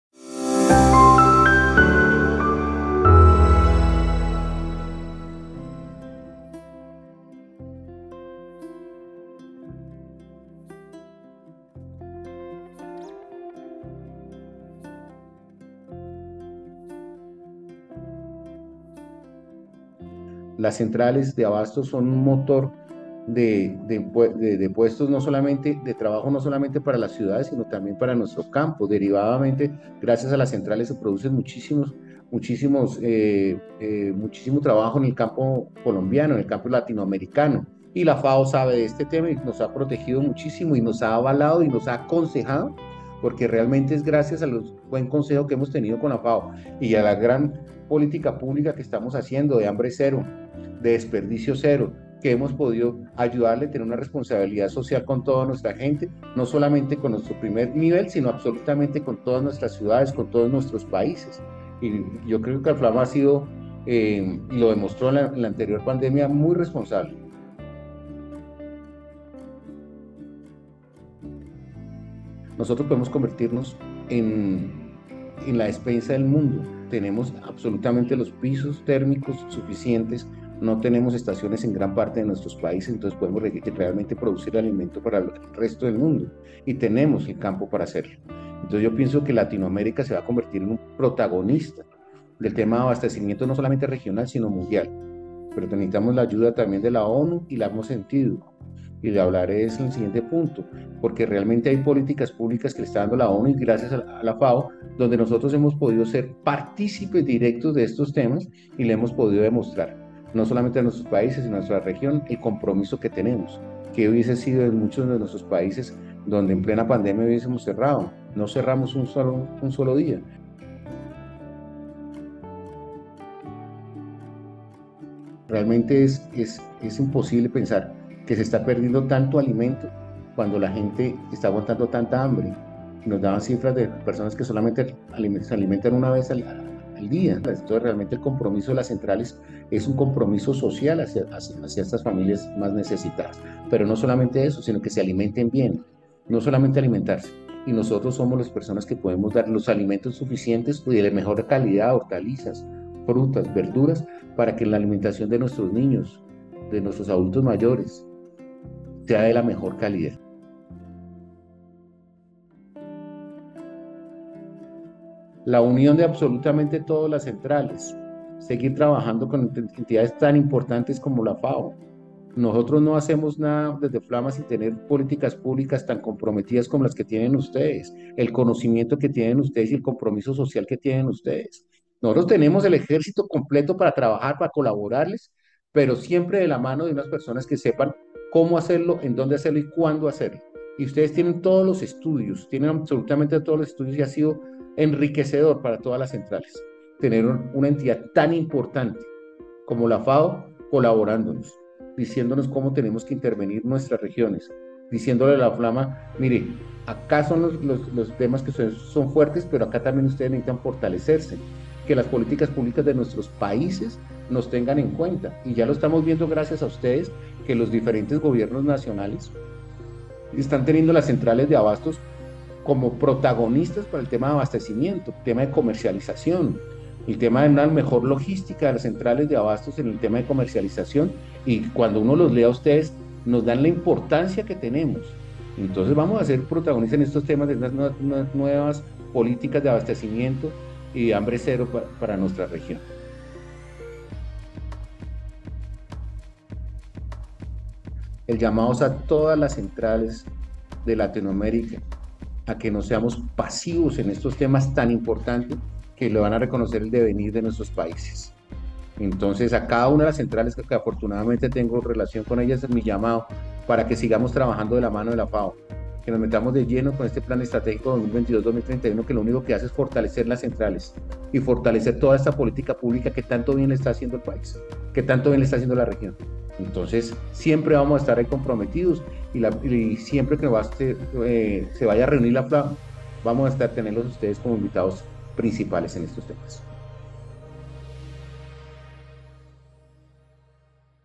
Mercados Tradicionales de Alimentos – Estimulando el Acceso a Alimentos Saludables
Entrevista a Javier Salcedo, Presidente de FLAMA (Federación Latinoamericana de Mercados de Abastecimiento), quien se refiere a las ventajas, el rol y el aporte que desempeñan los mercados tradicionales y las centrales de abasto en la región. En la ocasión, da cuenta de la importancia estratégica de la alianza FAO-FLAMA en materia de intercambio de experiencias y el desarrollo de capacidades técnicas, en especial ante los desafíos de América Latina y el Caribe.